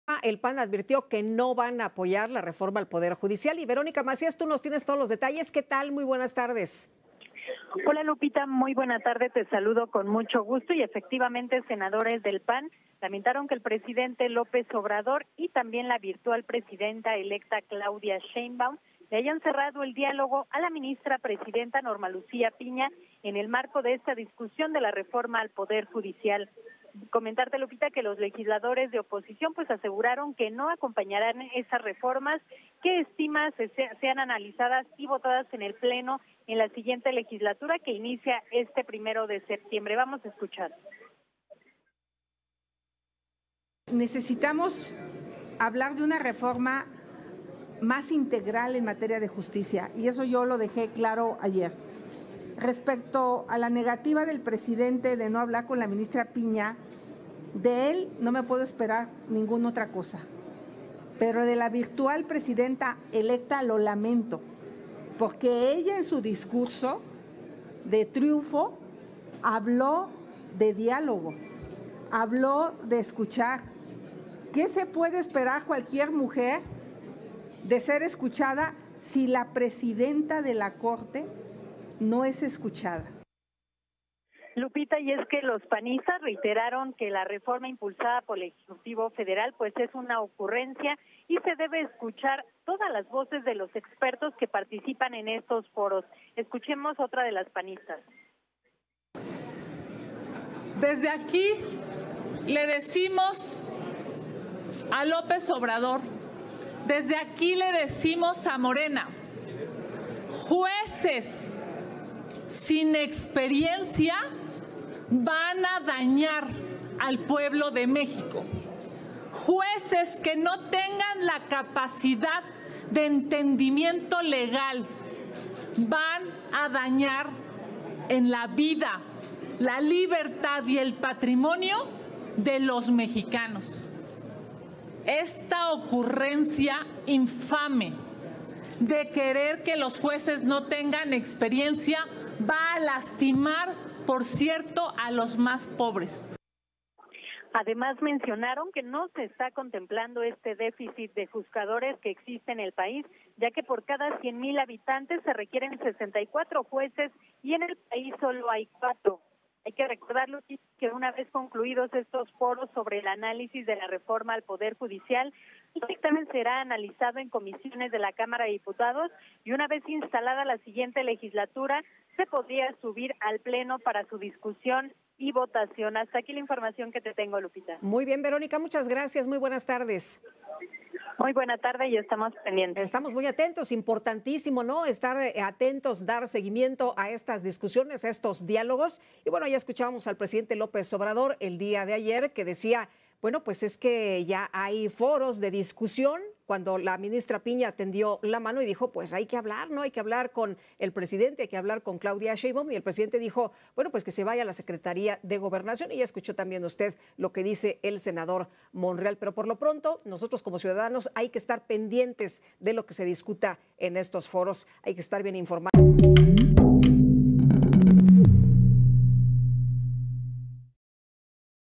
¡Senadores del PAN dicen NO!Niegan apoyar reforma al PJ y lamentan rechazo de diálogo con Norma Piña
Xóchitl Gálvez, ex candidata presidencial de la coalición Fuerza y Corazón por México, responde al presidente Andrés Manuel López Obrador durante el cuarto Diálogo nacional sobre la reforma al Poder Judicial en Chiapas. Gálvez defiende la importancia de la experiencia en jueces, magistrados y ministros de la Suprema Corte, contrastando con la postura del presidente. Propone una transformación del Poder Judicial sin odios ni venganzas, destacando que los defectos del sistema no justifican la arbitrariedad ni el abuso.